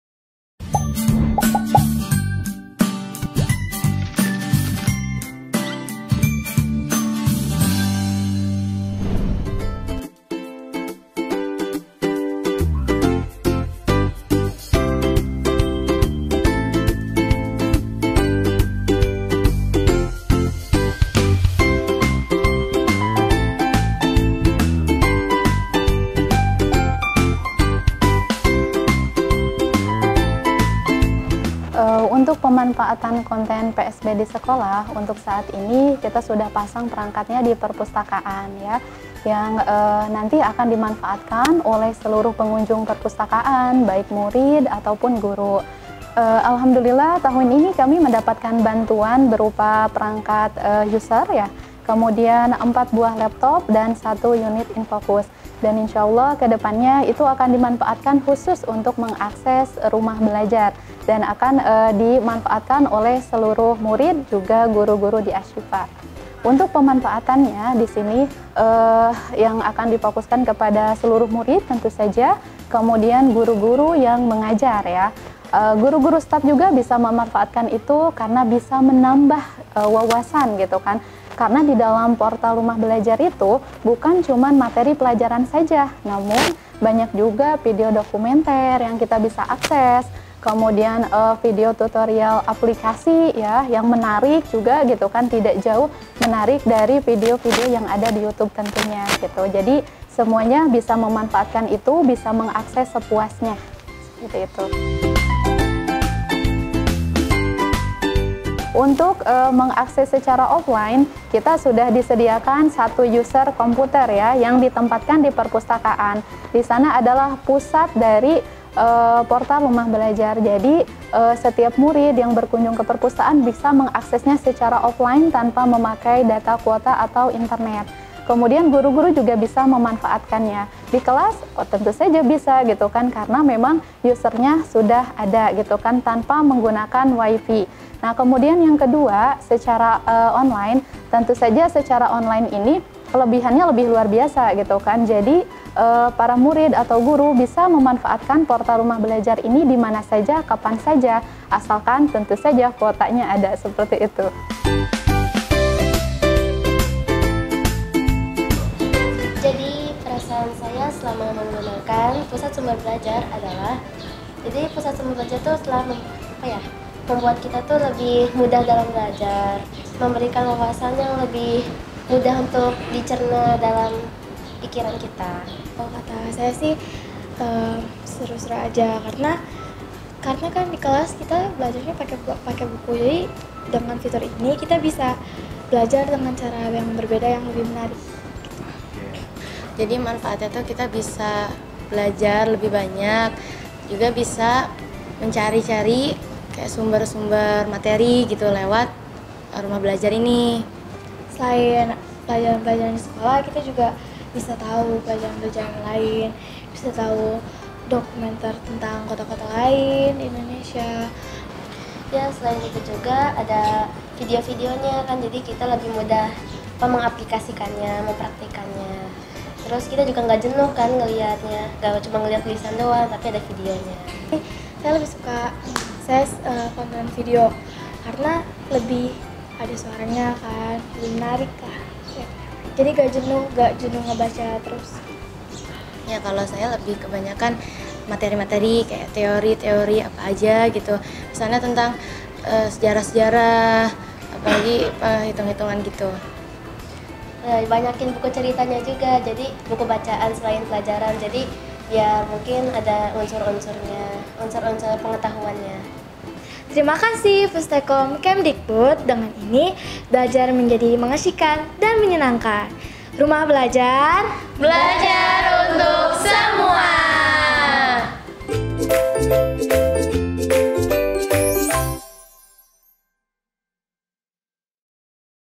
Pemanfaatan PORTAL RUMAH BELAJAR Di SMPIT As Syifa Boarding School
PortalRumahBelajar dokumentasi pemanfaatan portal rumah belajar sebelum terjadi pandemi covid 19, dokumentasi oleh tim ACMC Assyifa Al Khoeriyyah